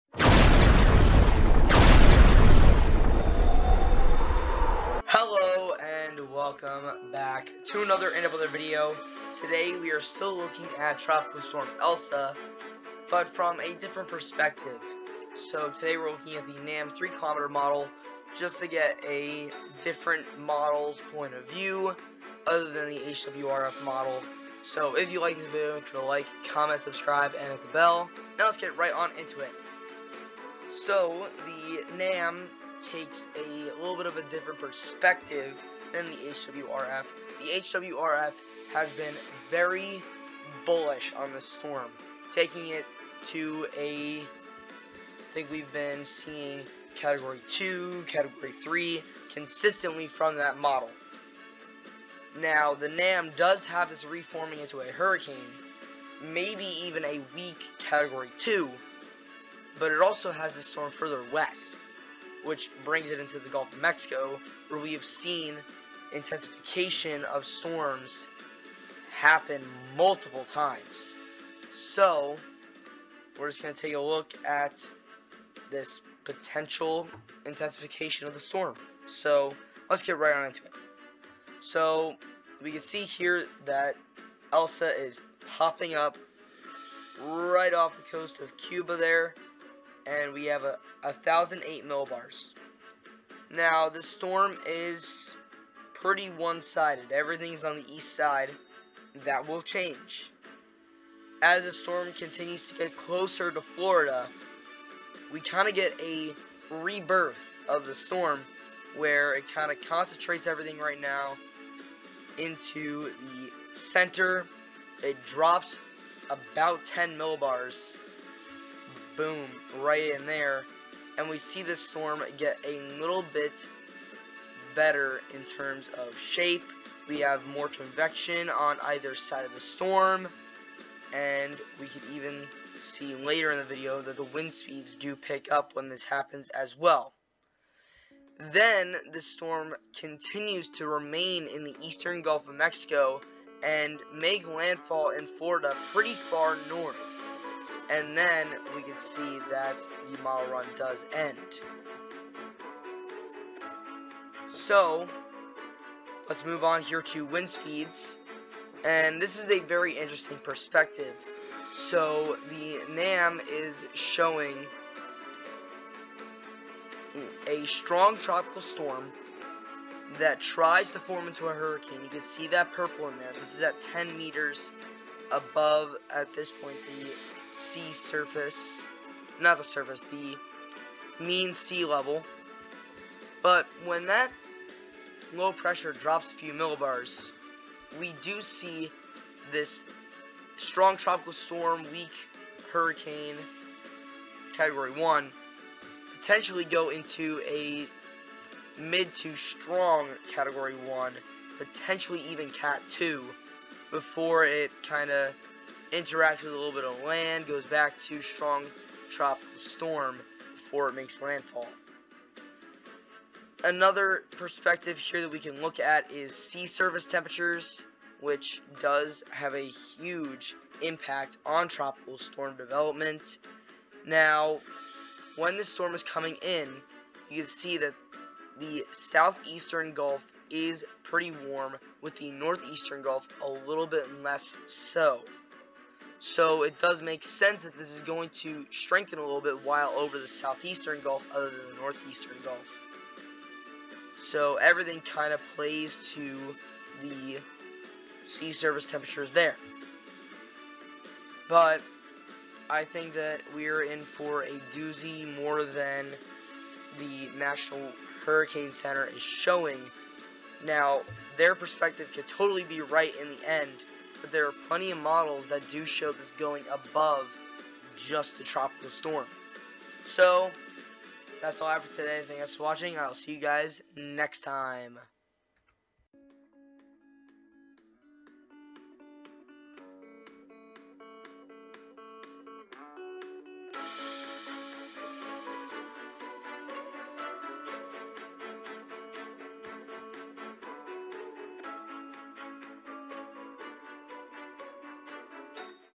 Another Perspective On Elsa 7/5/21
In this video, I take a look at Elsa from the perspective of the NAM 3km model.